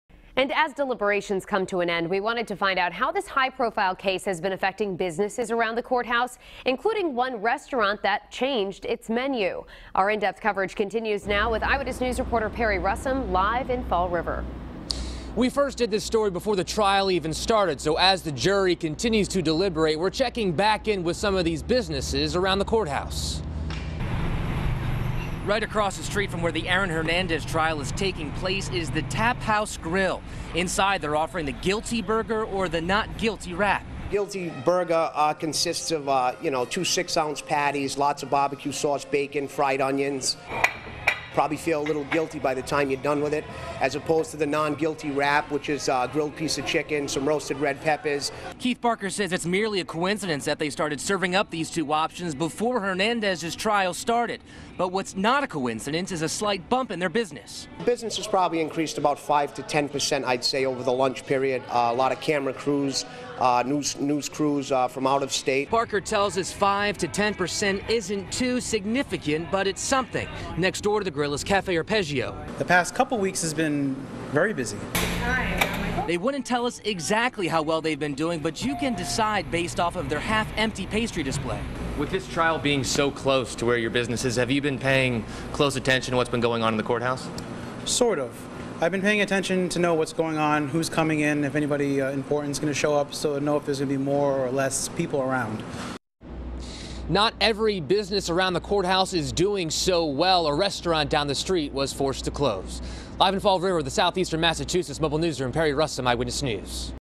Hernandez Trial Impacting Businesses
The Aaron Hernandez trial is having an effect on the restaurants located around the courthouse.